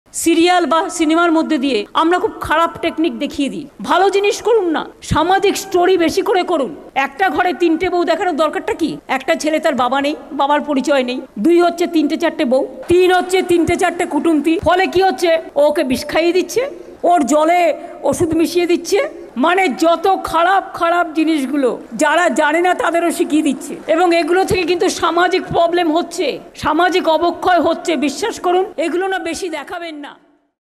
West bengal CM Mamata Banerjee speaks about serials
West Bengal chief minister Mamata Banerjee on Friday said her government will soon appoint a nodal officer in each district to check, along with the police, whether any TV program can upset communal harmony.
West Bengal chief minister also expressed her displeasure over some “negative” aspects that have become the fodder of most of the TV serials. “These days, television serials mean multiple marriages of a person or conspiring relatives. I request all to think about this issue,” she remarked.